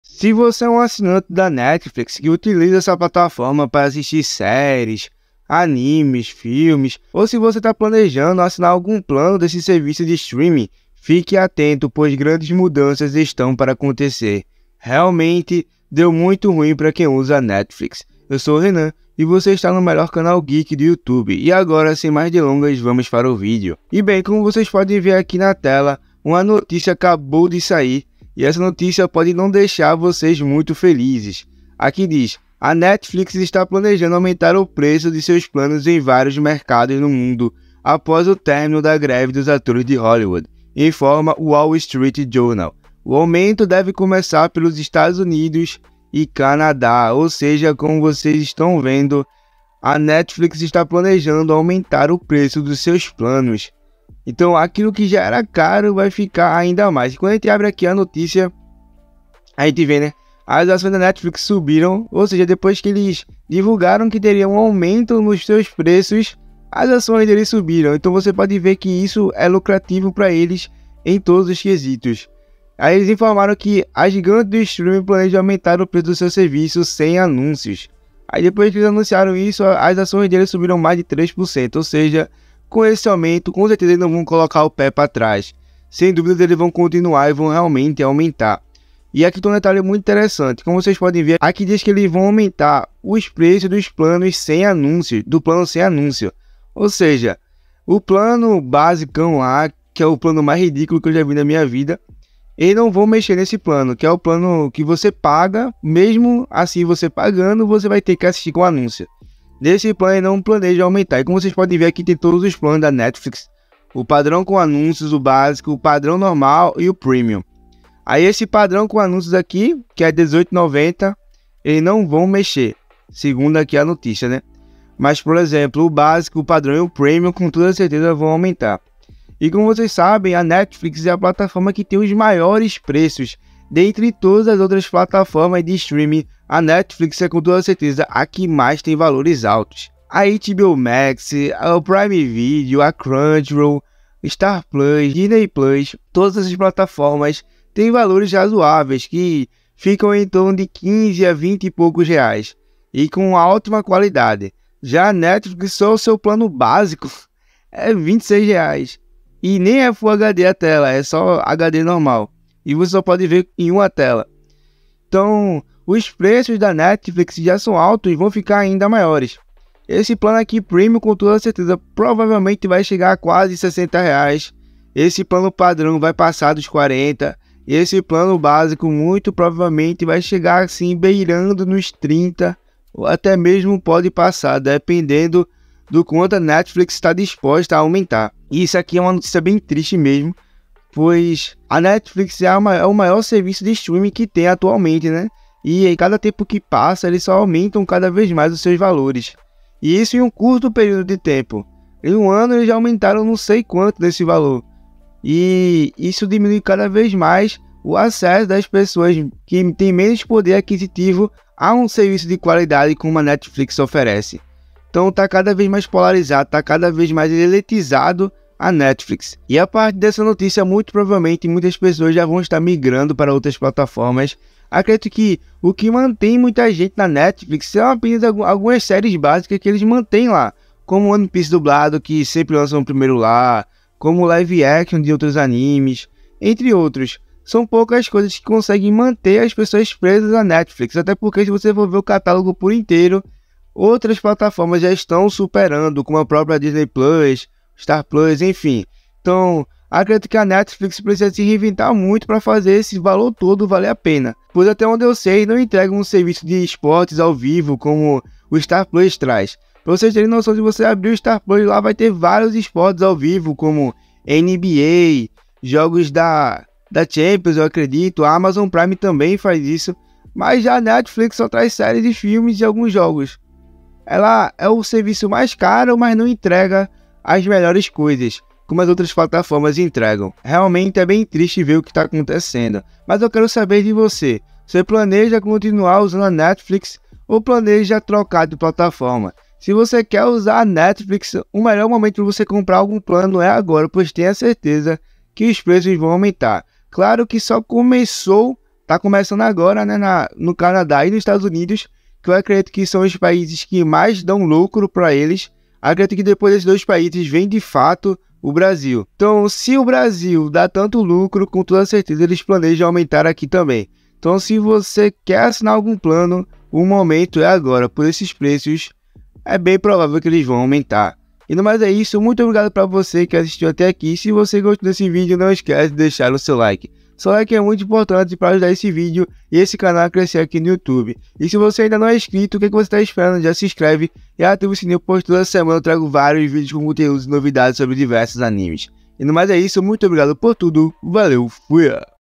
INACREDITÁVEL! DEU MUITO RUIM PRA QUEM USA A NETFLIX! GRANDES MUDANÇAS ESTÃO CHEGANDO...?
Se você é um assinante da Netflix, se prepara pois as coisas não estão boas...